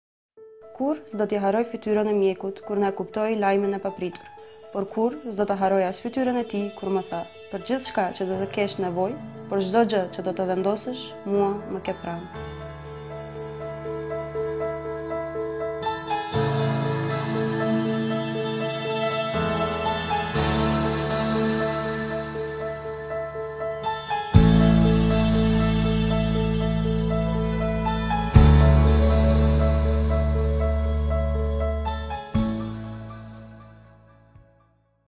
Слобода е кога самата одлучуваш! (видео 5)
Кампањата „Слобода е кога самата одлучуваш“ информира за придобивките што ги носи новиот закон за прекинување на бременоста.

Усвоен во мај 2019 година, новиот закон значително ја унапредува репродуктивната слобода кај нас затоа што придонесува кон поквалитетна и подостапна абортусна грижа и послободен и побезбеден репродуктивен живот за жените, мажите и семејствата во земјата.